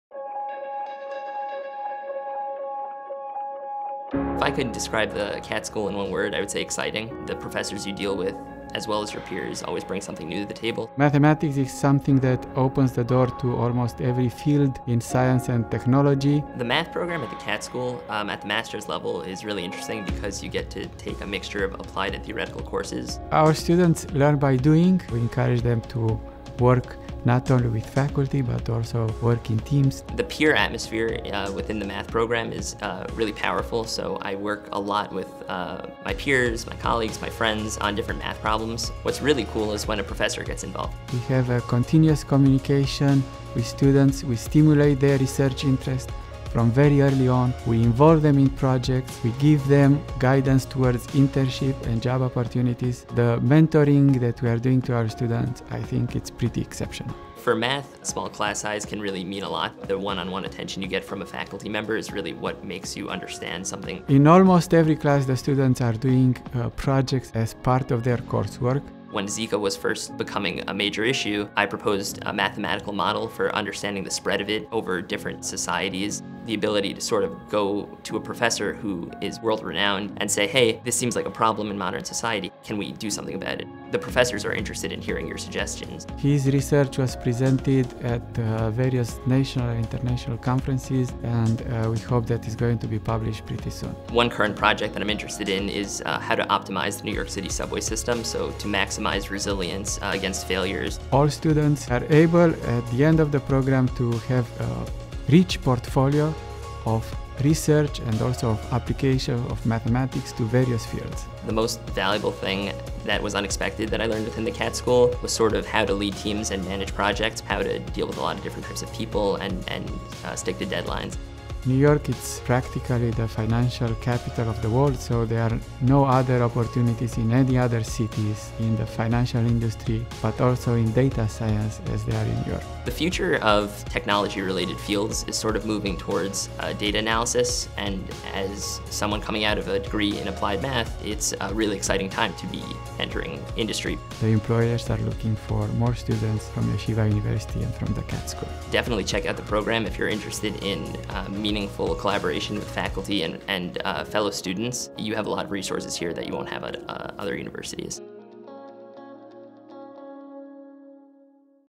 Mathematics | The Katz School at Yeshiva University
The Katz 30-credit master's in Mathematics is offered on campus in New York City.